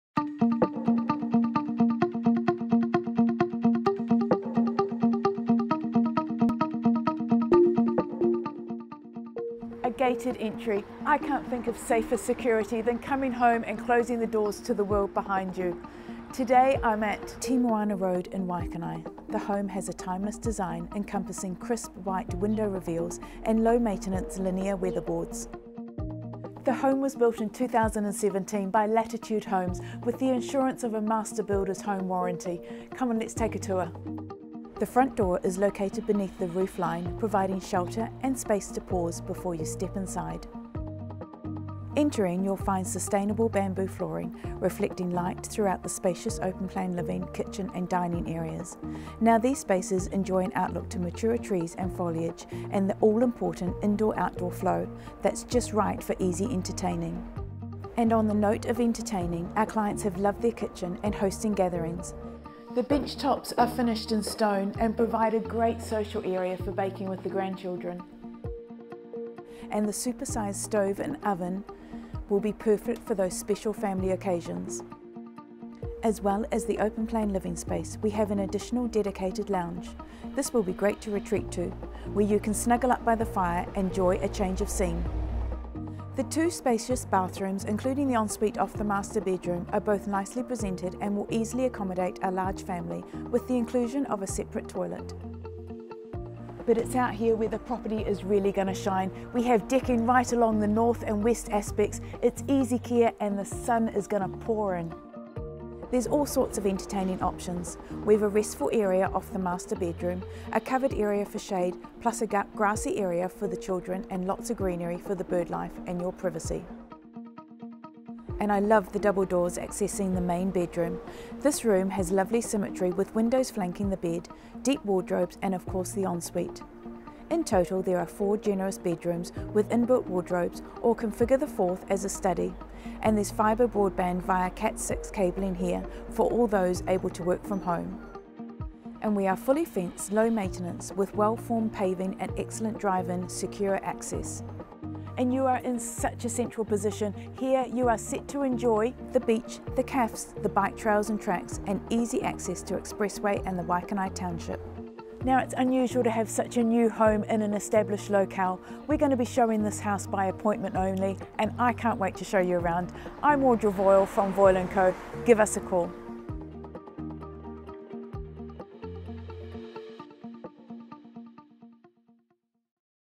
SOLD | 199a Te Moana Road, Waikanae
Almost new and waiting for you
199a  Te Moana Road, Waikanae
BEO $990,000

Come home and close the doors to the world behind you - the gated entry to this home provides safety and security. Showcasing timeless design with crisp white window reveals and low maintenance Linea weatherboards, this home was built in 2017 by Latitude Homes and comes with the assurance of a master build warranty.

Mike | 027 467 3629 or Audra | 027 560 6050